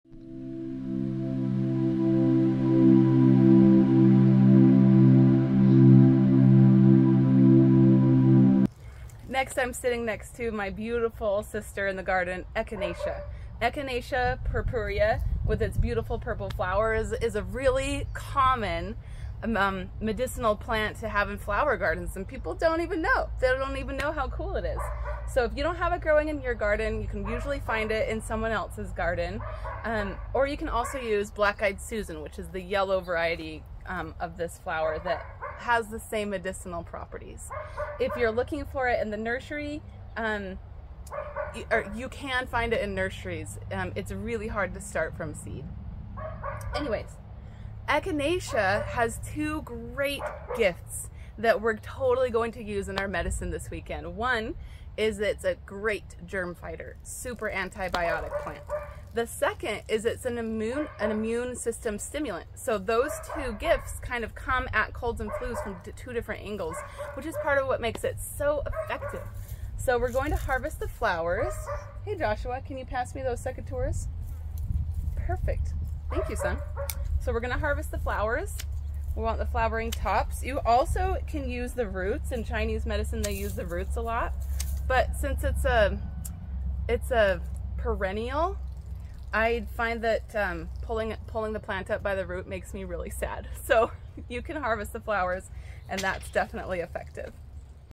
Echinacea uses for Medicine Badge Class
This immune-boosting plant ally is useful in germ-fighting and prevention herbal formulas. We will use it in gummies and Fire Cider.

It is one of more than 20 plants that are used in the Natural Medicine Badge from Permies.com SKIP program: a great place to learn herbal medicine for FREE.

For a guide on how to complete this badge and make all the required herbal preparations, check out my $25 class: